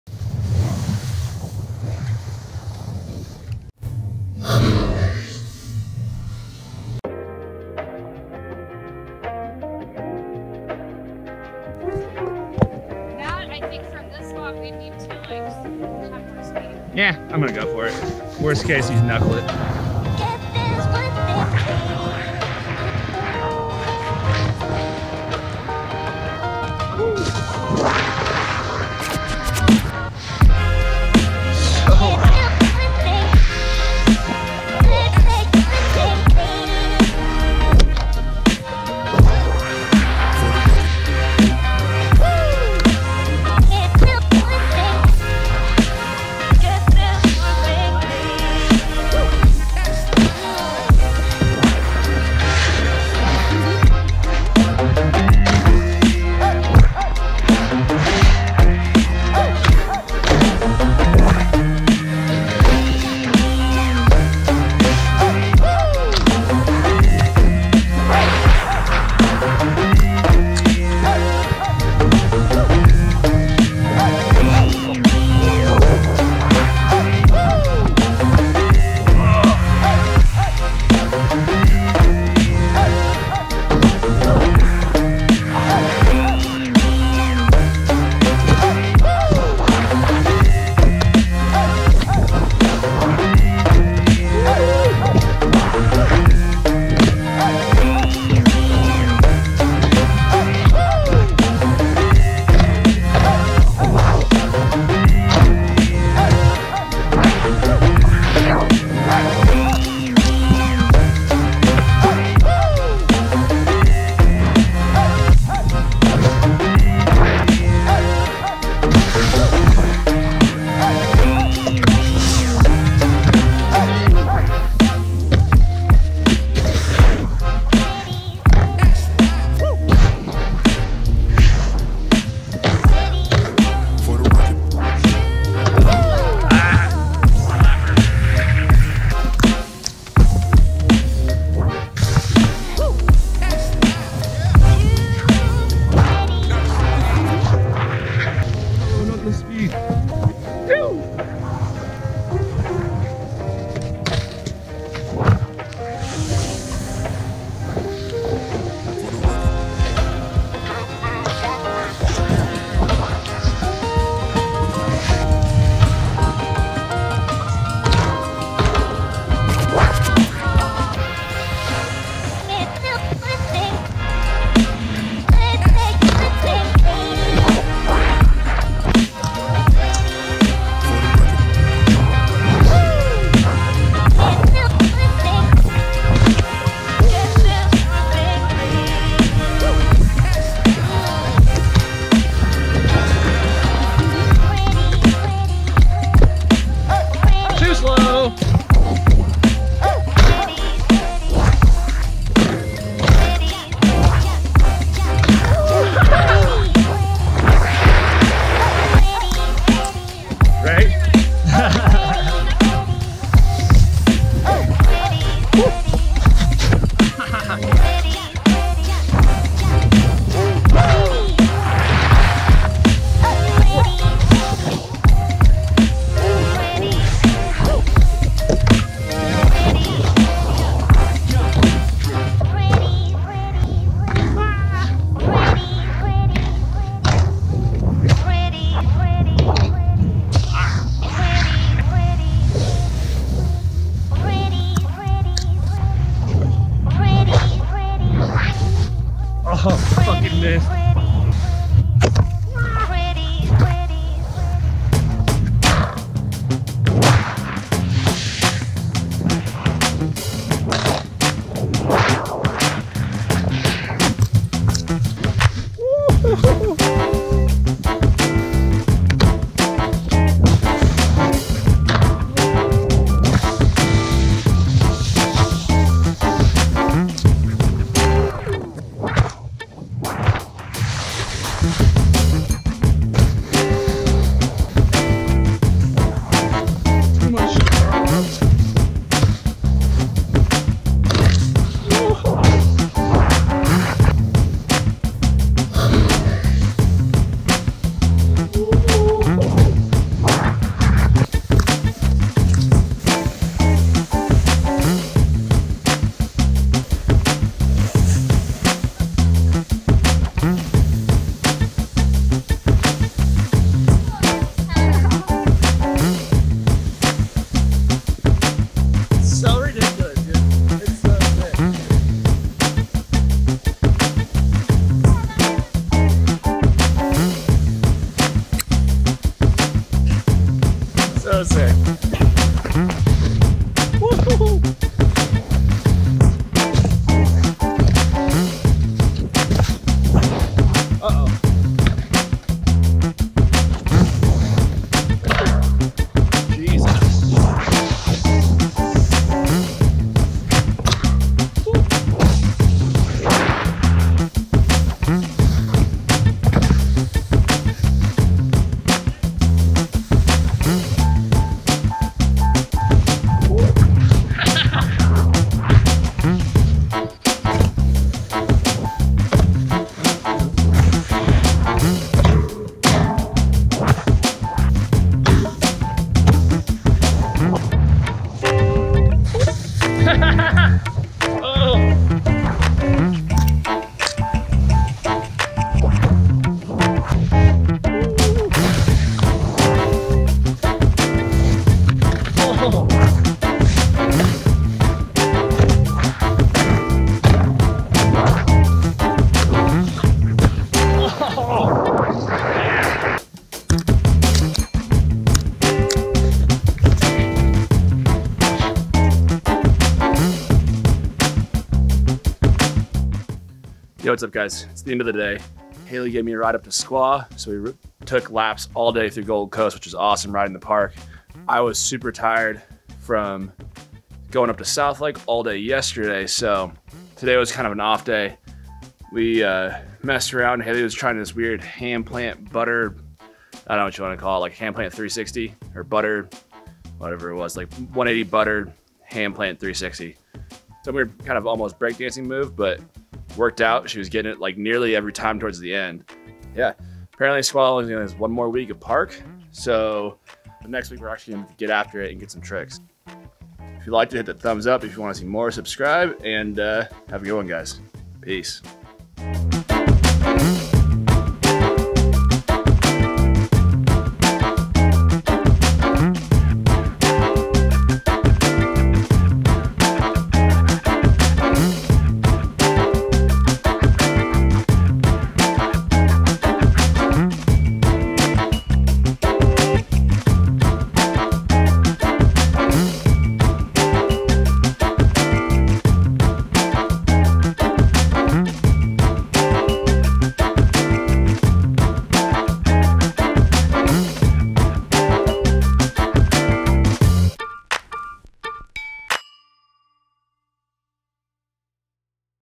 Squaw Valley Park in June
6-2-19 : The 2nd to last weekend of park at Squaw. The small line was perfect for a recovery day after spending the entire day prior in South Lake. The step up/over and hip step up/over transitions were a bit too short to be enjoyable - so they were used as side hits.